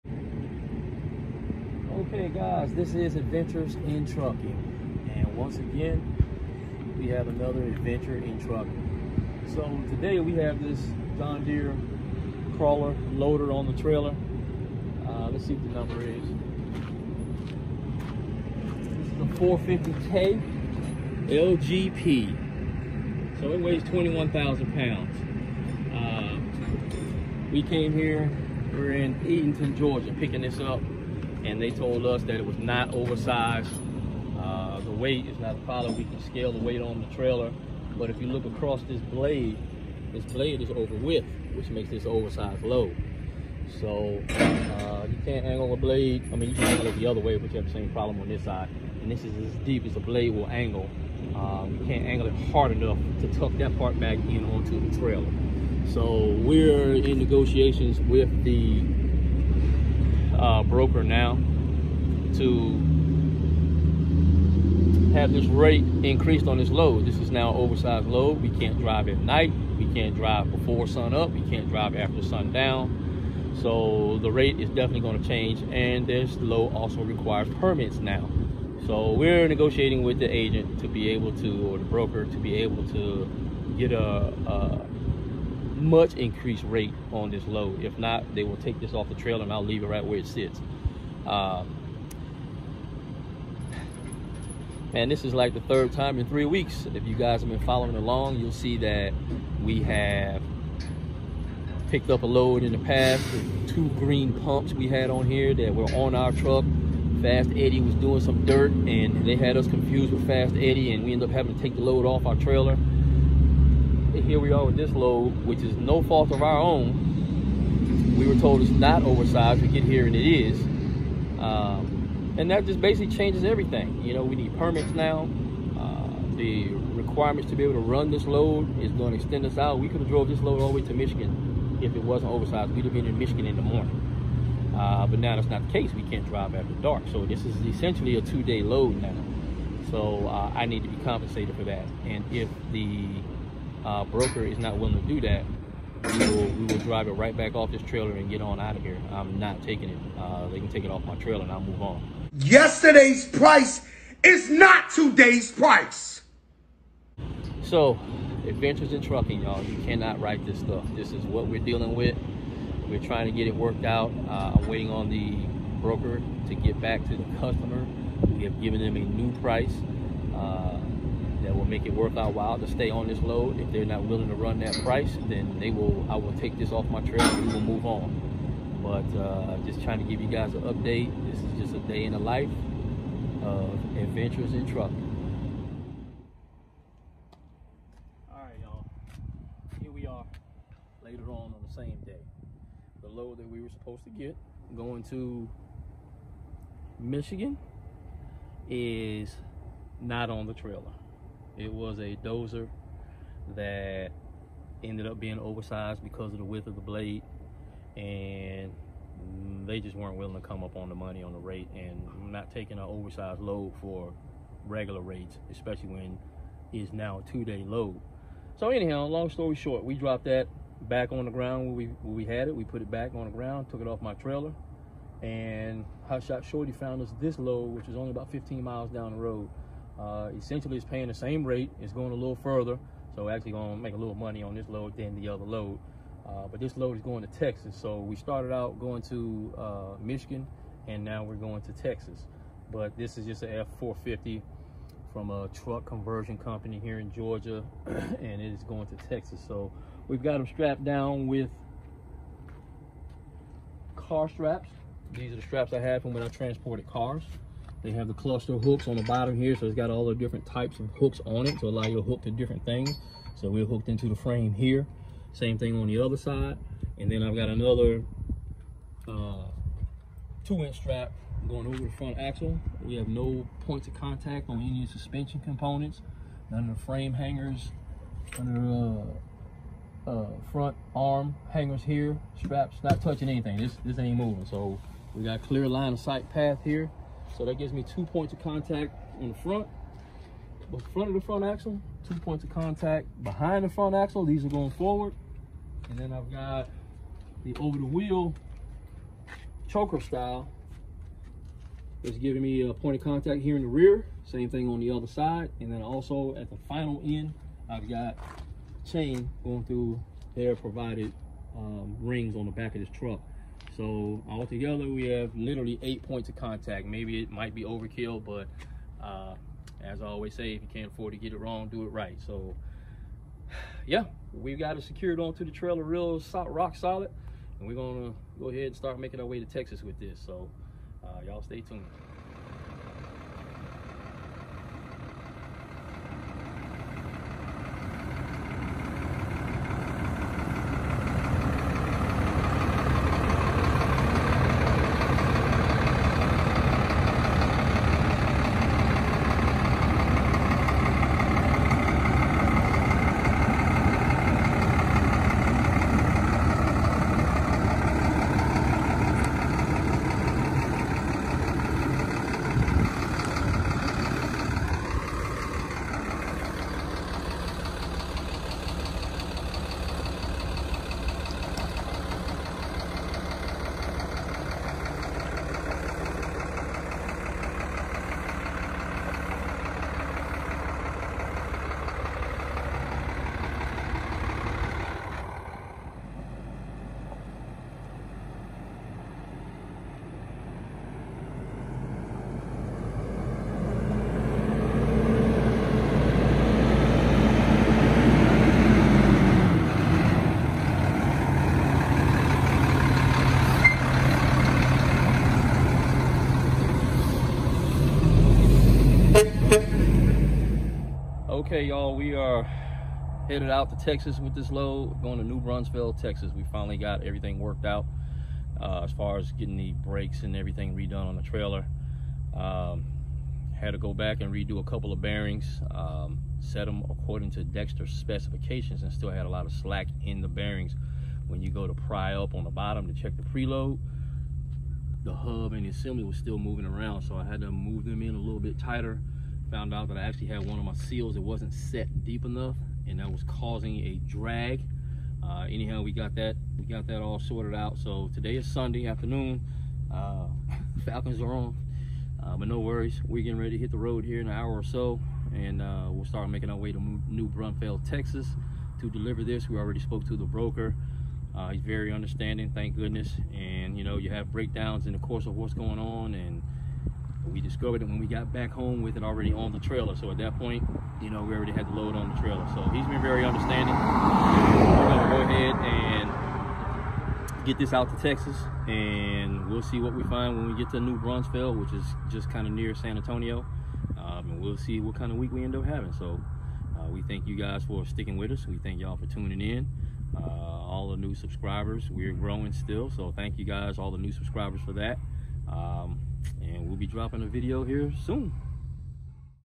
Another Load Gone Wrong?! | CDL Hotshot | Bigshot
In todays video we have part 1 of 2 of our week in trucking!